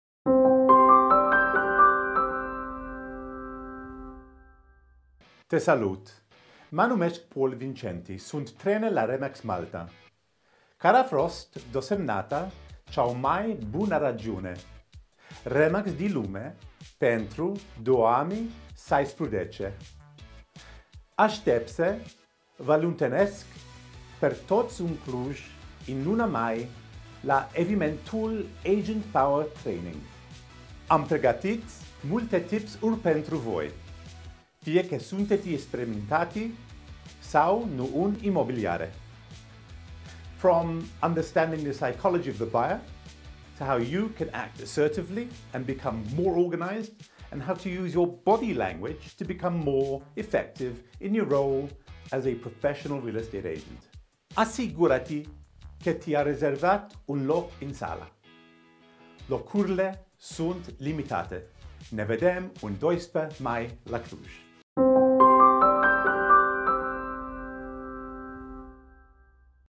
A message to Romania from Paul Vincenti
I'm looking forward to seeing everyone in May 2017